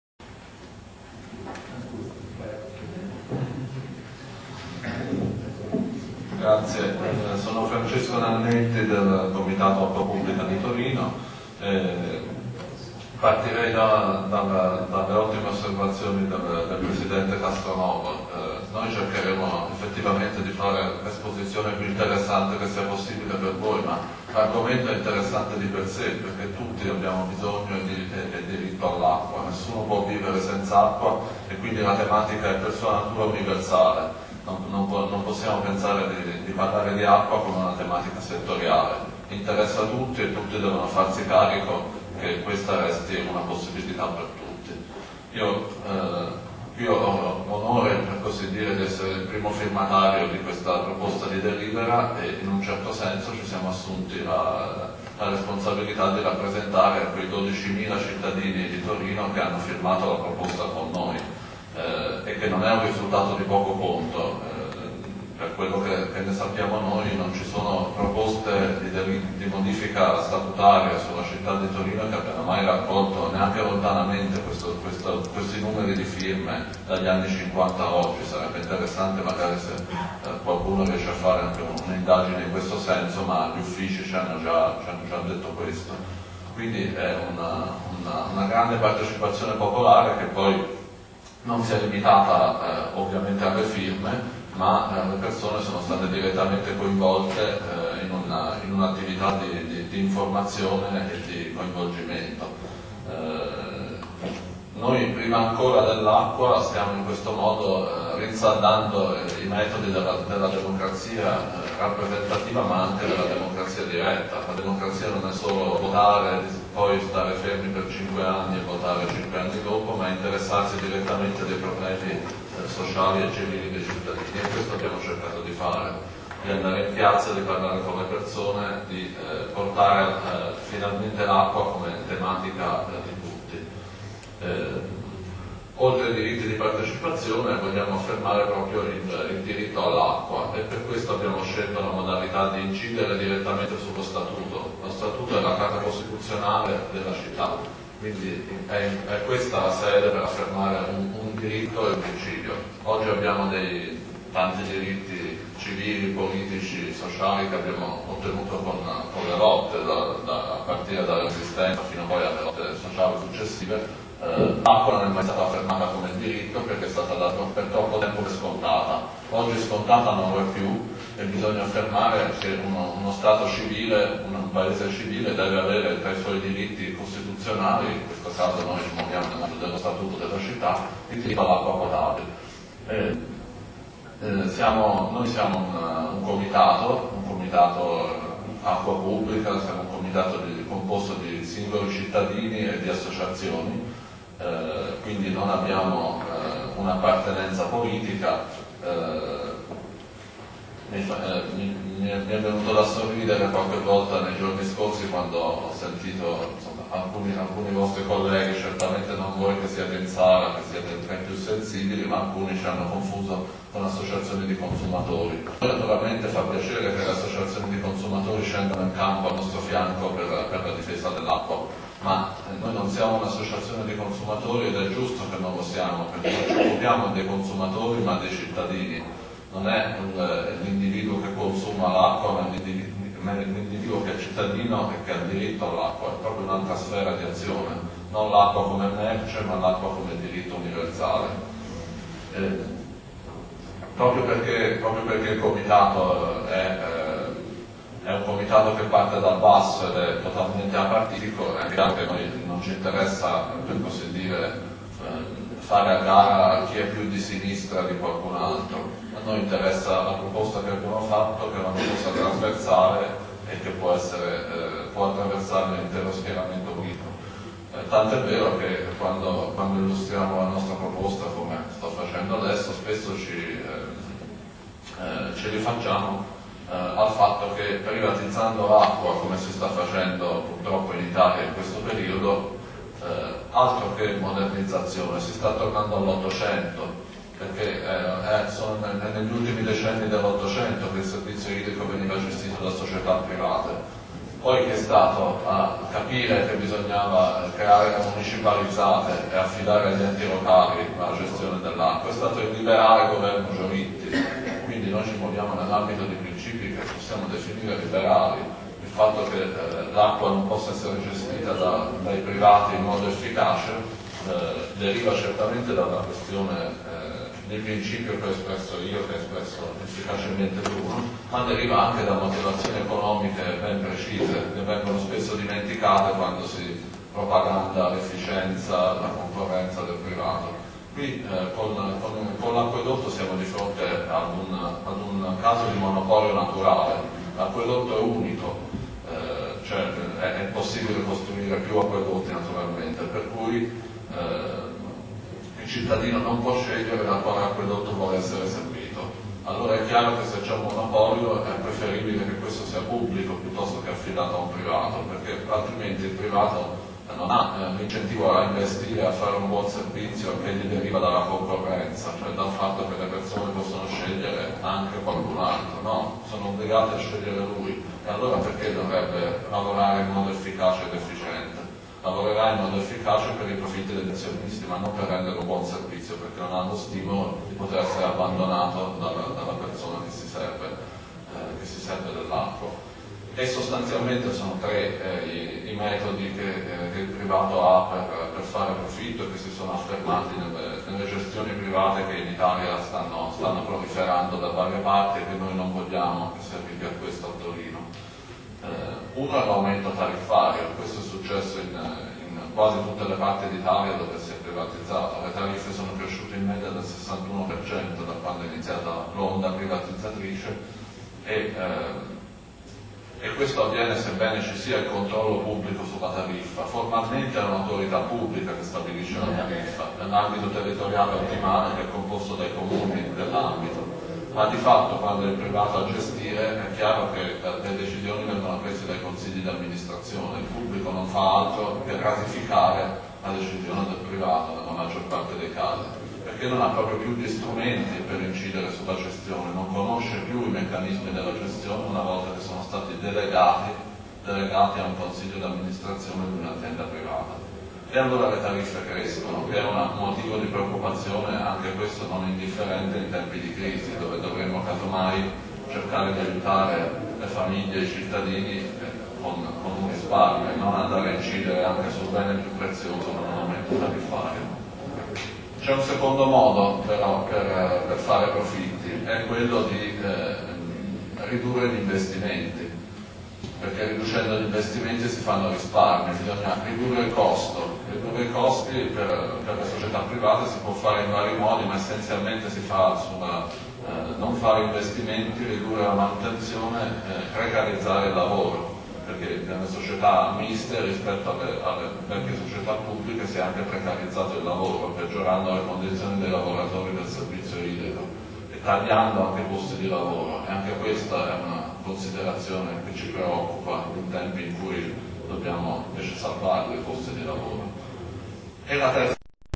Diritto di Tribuna - Parte2 - Francesco
Diritto di Tribuna - Parte 2: Intervento di Francesco del Comitato Acqua Pubblica. Illustrazione della delibera.